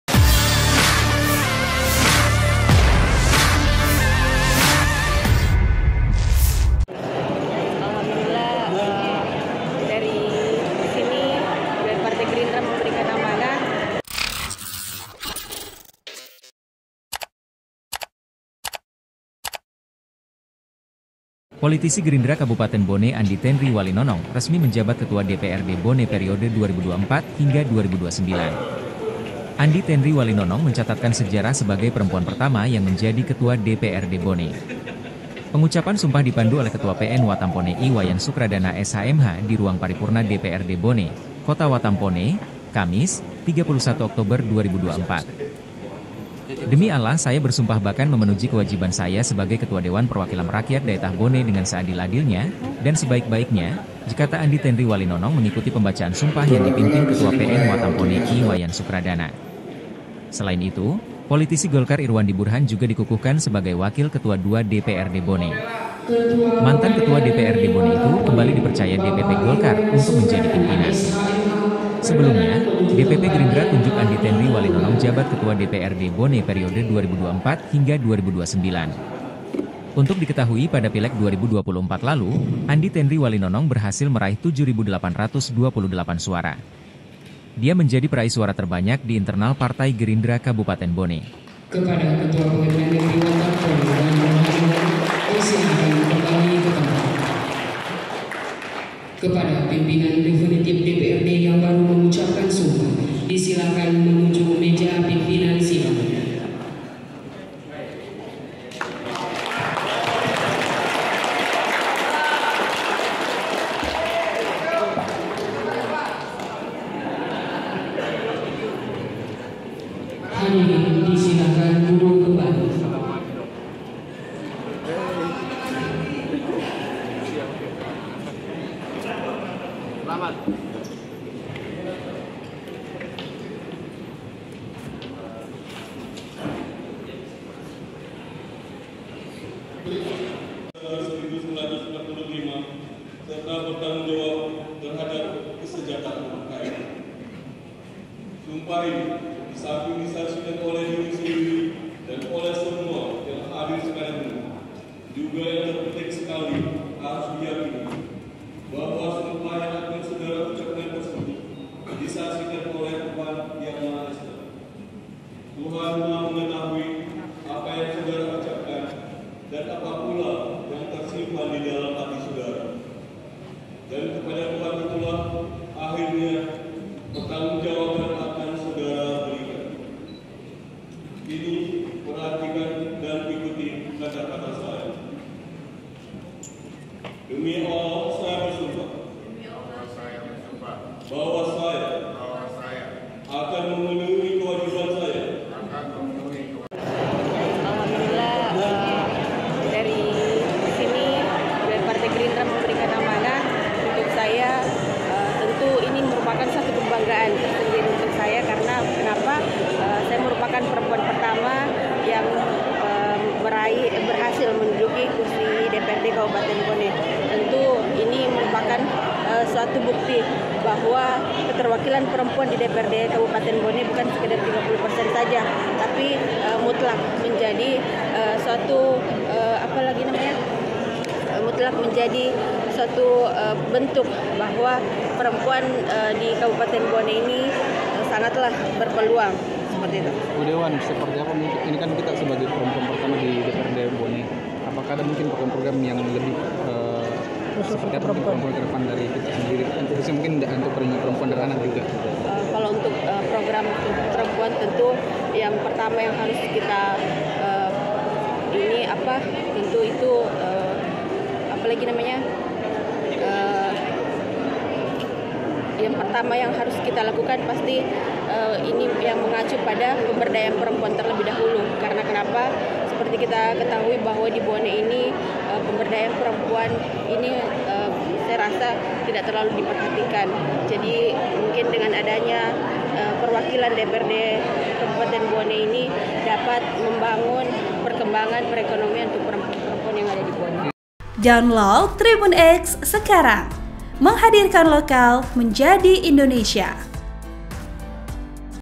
Andi Tenri Walinonong Catatkan Sejarah, Perempuan Pertama Jabat Ketua DPRD Bone
TRIBUNBONE.COM - Politisi Gerindra Kabupaten Bone Andi Tenri Walinonong resmi menjabat Ketua DPRD Bone Periode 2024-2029.
Andi Tenri Walinonong mencatatkan sejarah sebagai perempuan pertama yang menjadi Ketua DPRD Bone.
Pengucapan sumpah dipandu oleh Ketua PN Watampone I Wayan Sukradana SH MH di ruang paripurna DPRD Bone, Kota Watampone, Kamis (31/10/2024).
"Demi Allah saya bersumpahbakan memenuji kewajiban saya sebagai Ketua Dewan Perwakilam Rakyat Daetah Bone dengan seadil-adilnya, dan sebaik baiknya," kata Andi Tenri Walinonong mengikuti pembacaan sumpah yang dipimpin Ketua PN Watampone I Wayan Sukradana.
Selain itu, politisi Golkar Irwandi Burhan juga dikukuhkan sebagai Wakil Ketua II DPRD Bone.
Mantan Ketua DPRD Bone itu kembali dipercaya DPP Golkar untuk menjadi pimpinan
Sebelumnya, DPP Gerindra Tunjuk Andi Tenri Walinonong Jabat Ketua DPRD Bone Periode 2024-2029
Untuk diketahui pada Pileg 2024 lalu, Andi Tenri Walinonong berhasil meraih 7.828 suara.
Dia menjadi peraih suara terbanyak di internal Partai Gerindra Kabupaten Bone.

editor video: rahmat roy
naskah: Wahdaniar
reporter: Wahdaniar

(TRIBUN-TIMUR.COM)